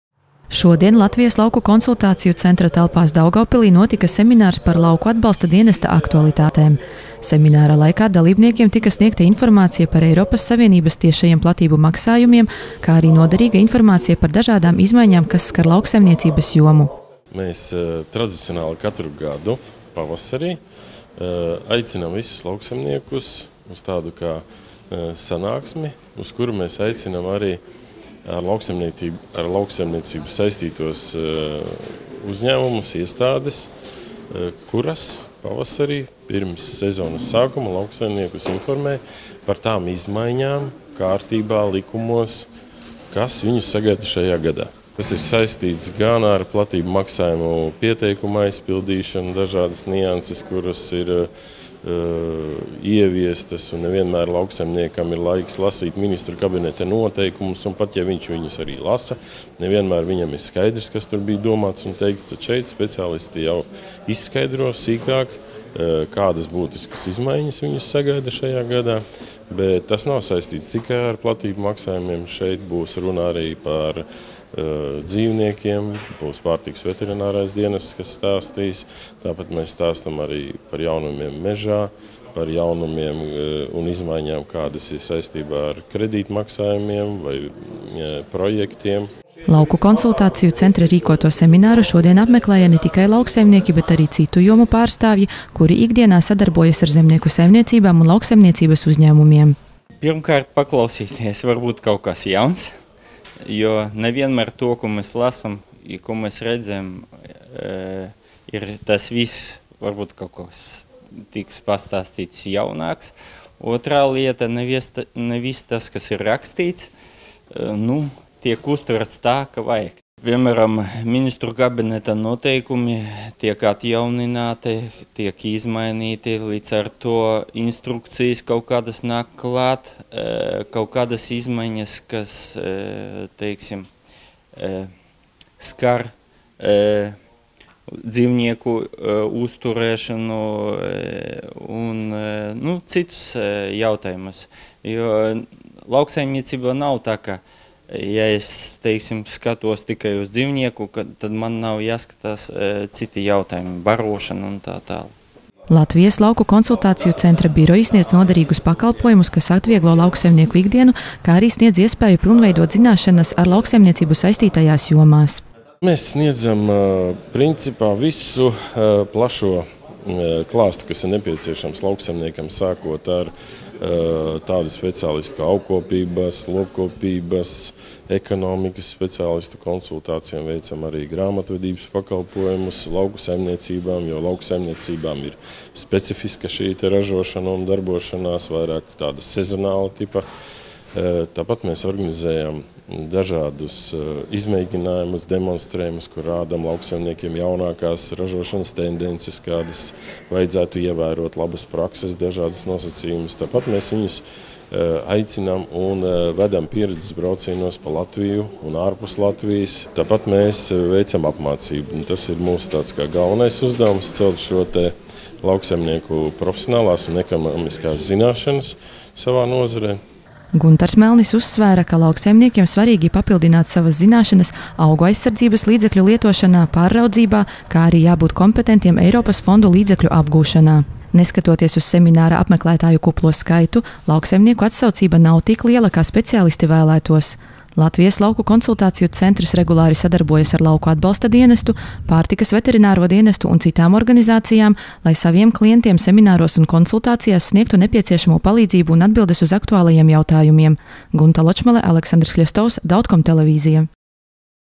Daugavpilī notika seminārs lauksaimniekiem
3. aprīlī Latvijas Lauku Konsultāciju centra Daugavpils birojā notika seminārs par Lauku Atbalsta dienesta aktualitātēm 2019. gadam. Seminārā dalībniekiem bija iespēja tikties ar LLKC, LAD un PVD pārstāvjiem, lai saņemtu jaunāko informāciju par dažādām izmaiņām, kas jau no šī gada skar lauksaimniecības nozari.